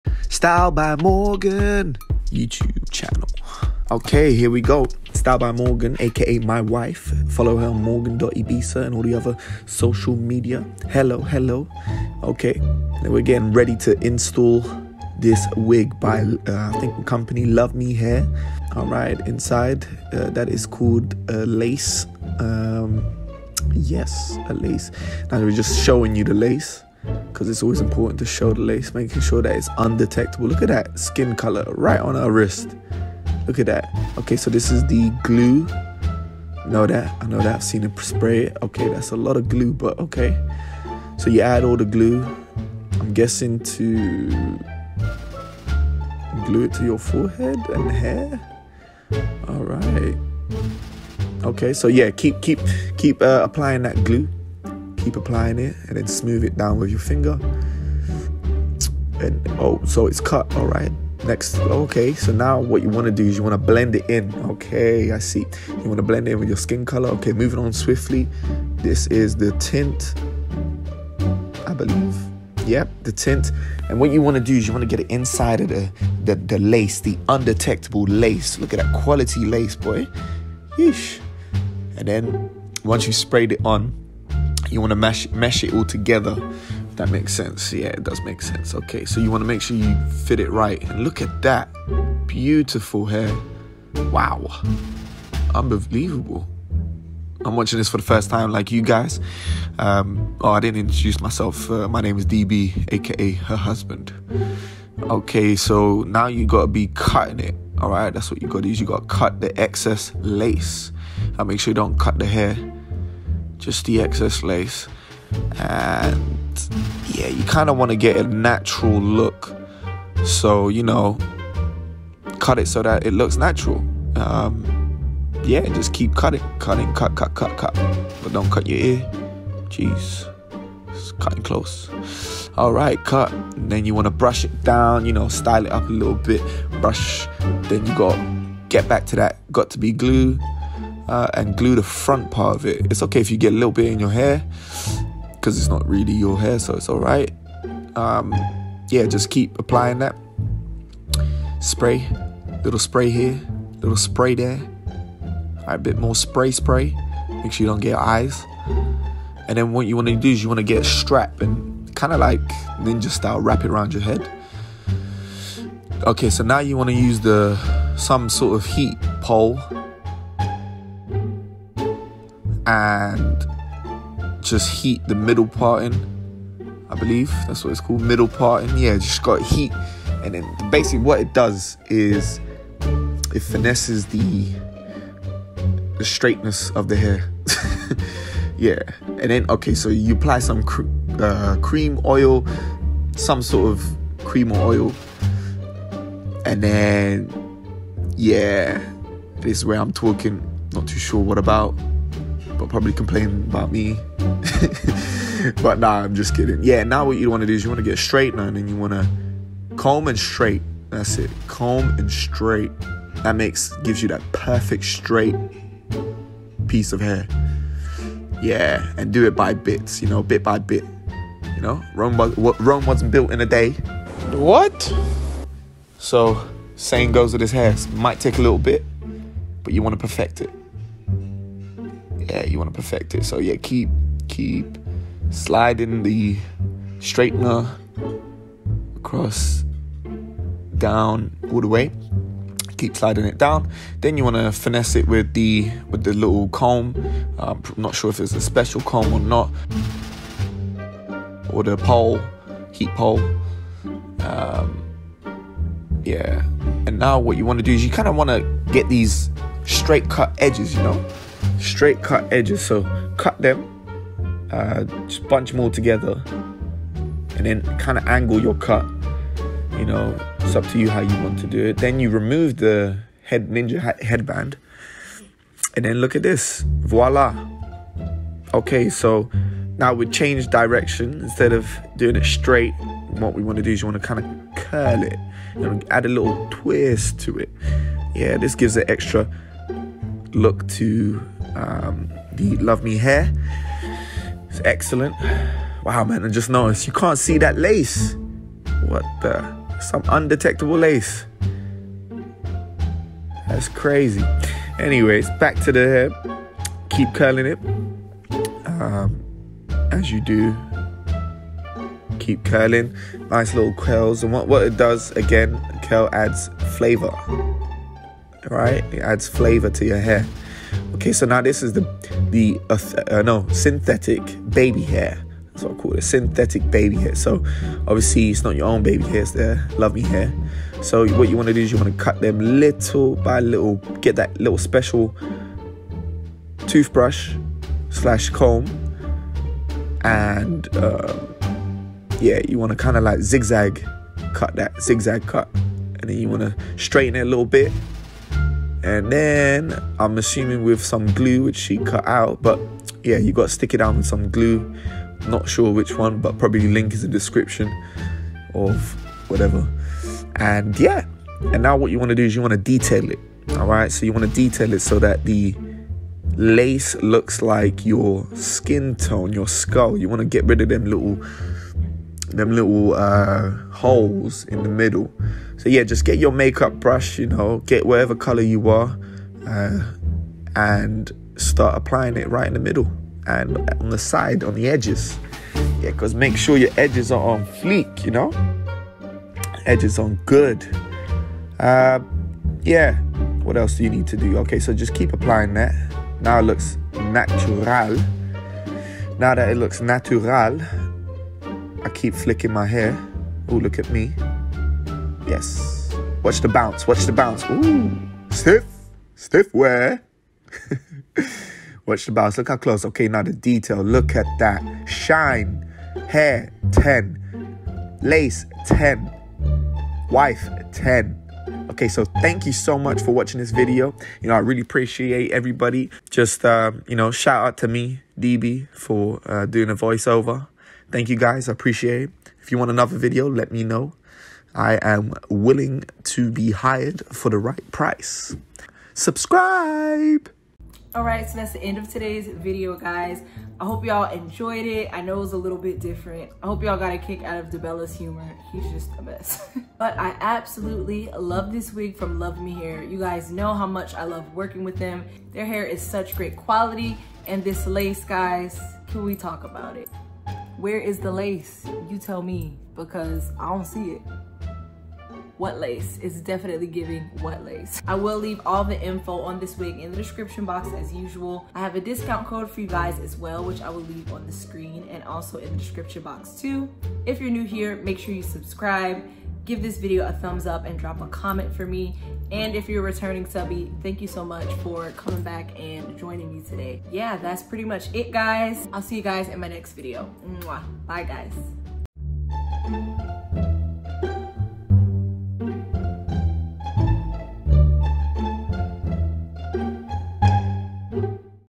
HUSBAND DOES MY WIG INSTALL VOICEOVER | FT. LUVME HAIR UNDETECTABLE LACE WIG
Hey babes! I hope you enjoy this funny video of my husband doing a voiceover for my wig install lol!

FREQUENTLY ASKED QUESTIONS:

AGE: 25
HEIGHT: 5'4
WEIGHT: 135 lbs
LOCATION: LONDON, U.K.
EDITING SOFTWARE: MOVAVI PRO